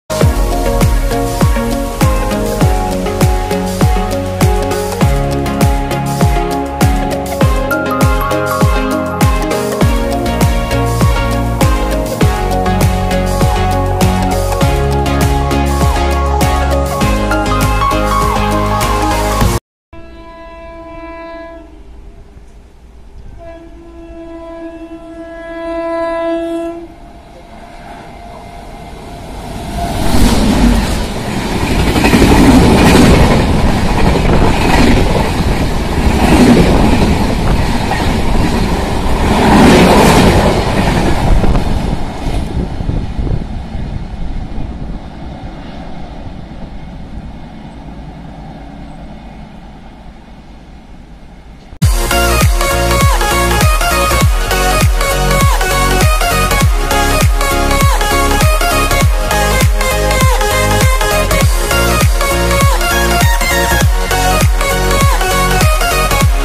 ECoR's Speedster Late Running 12278 Puri Shatabdi attacks Kulgachia with SRC HOG WAP 7 at MPS !!!
An hour Delayed 12278 Puri - Howrah Shatabdi Express glides through Kulgachia at flat 110 KMPH with 30760 Santragachi ( SRC ) HOG WAP 7 in lead !!!